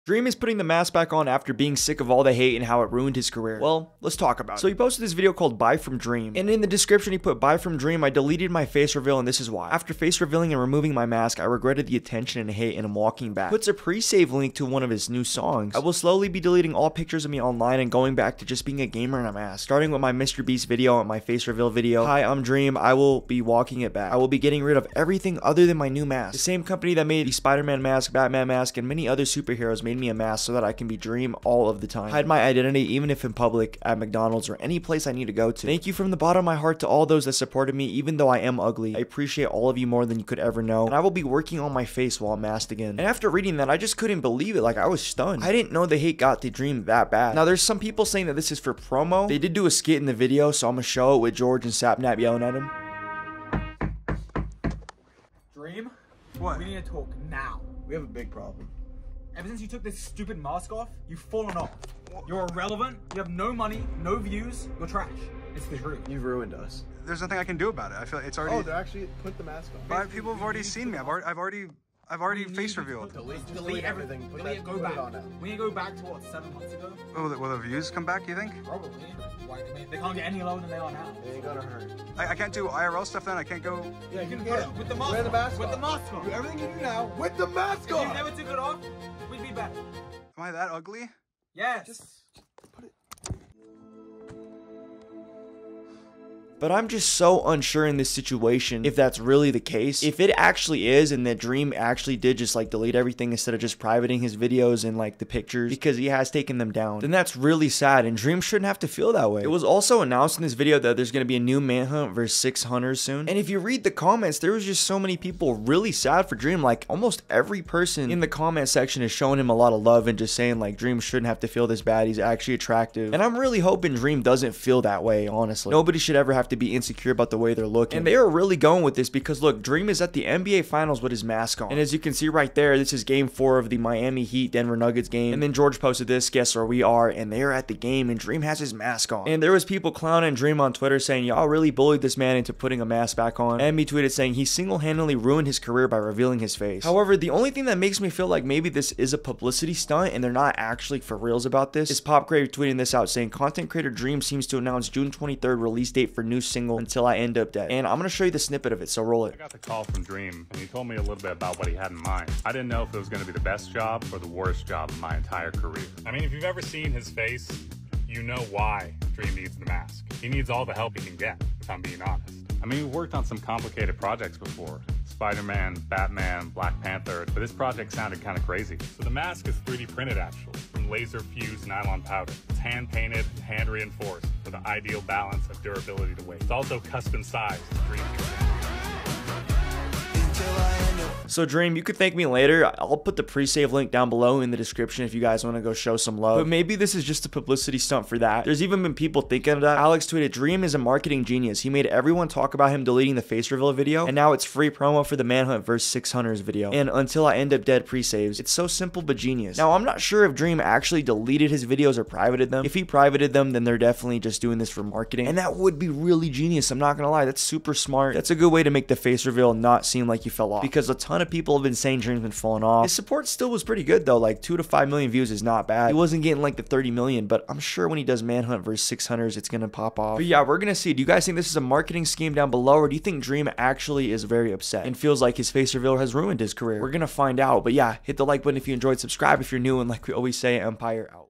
Dream's Face Reveal Ruined His Career... (Explained)
►Dream recently posted a video saying he will go back to wearing the mask full time and will delete any content with his face. Today we discuss the drama and if it's a possible PR stunt for his new song or if dream's actually hurt by the backlash he received.